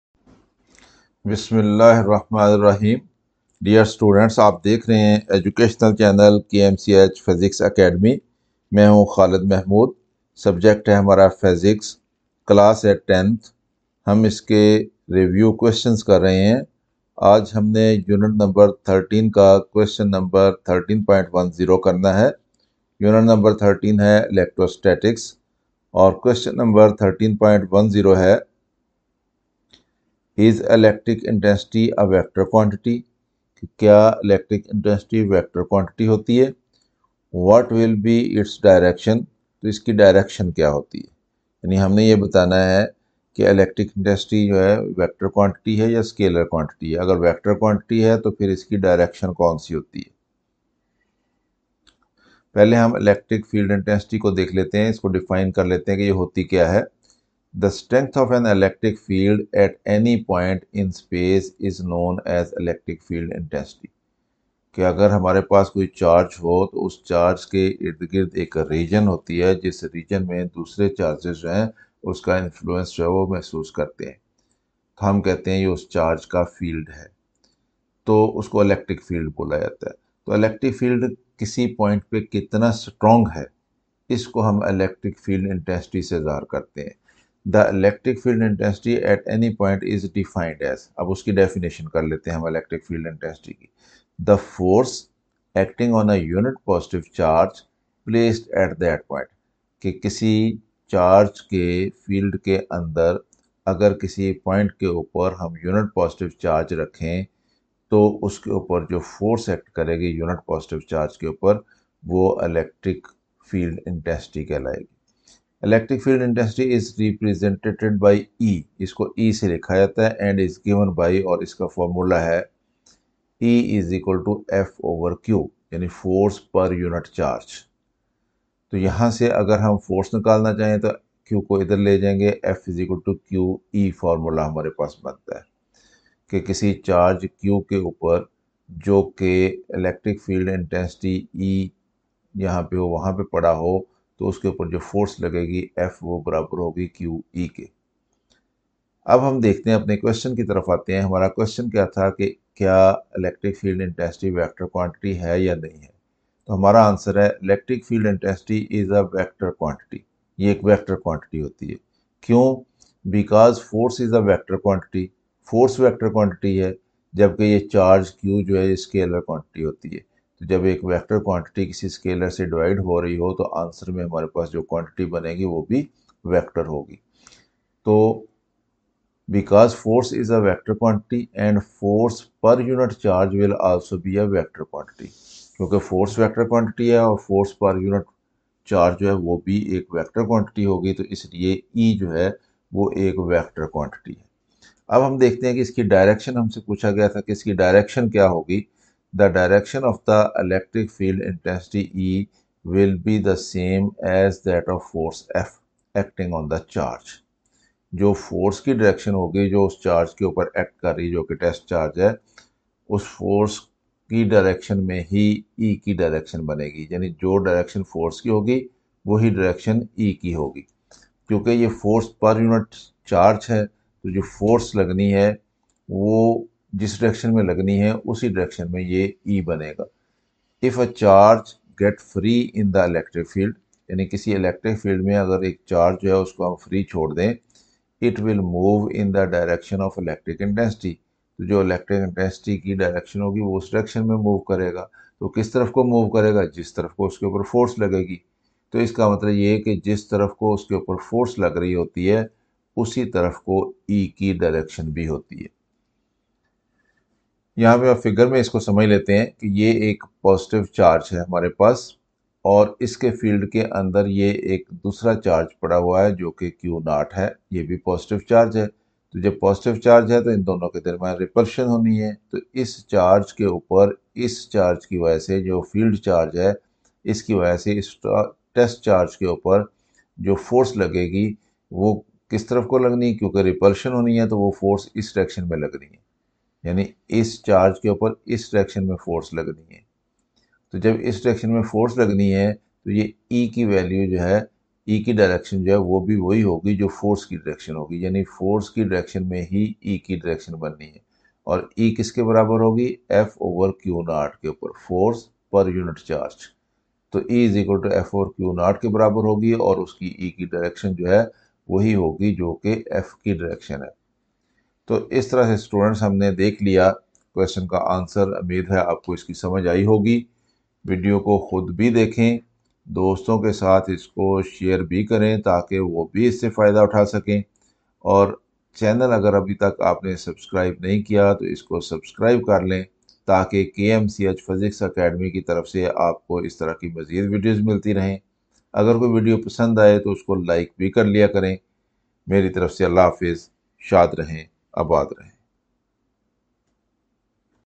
Q 13.10 | Unit 13 | Electrostatic Induction | Class 10 | Physics-X | km ch Physics Academy
Q 13.10  Is electric intensity a vector quantity? What will be its direction?